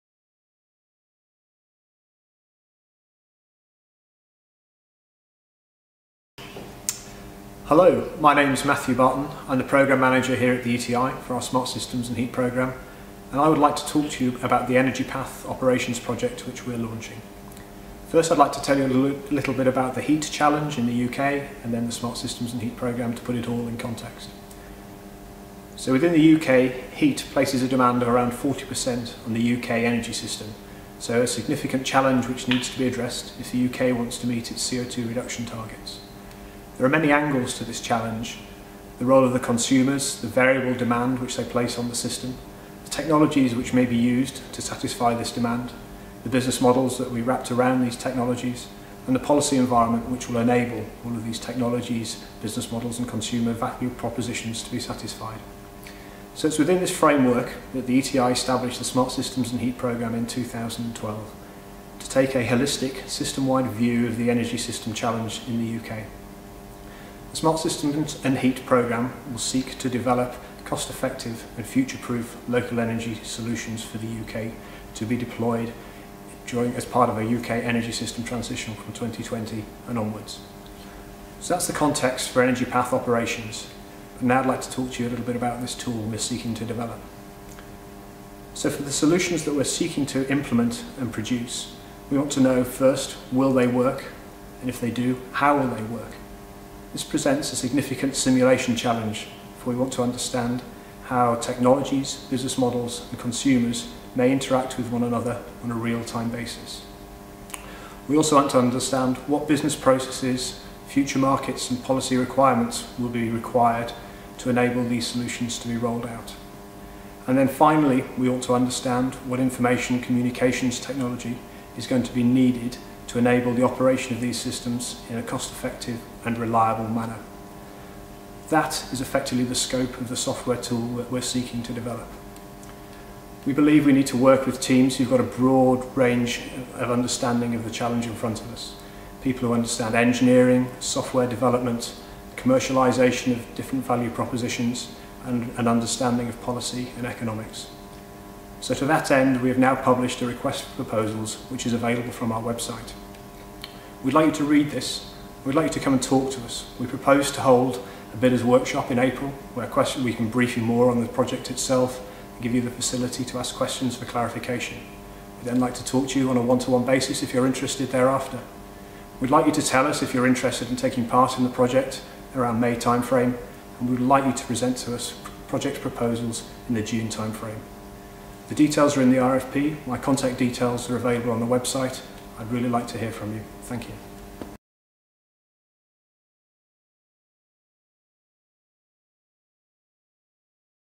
Matthew Barton SSH Programme Manager
Request for Proposals - Energy Path Operations.
ETI seeks partners to develop innovative software tool to simulate future local energy systems.